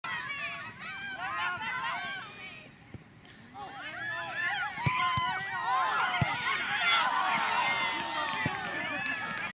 Blackhills FC G'00 - Spring 2014 - Game 5 - Spokane Shadow - Great Save from Trinity
Without Trinity this match might have been 7, or 8, to nil.  This is one of her better saves of the night.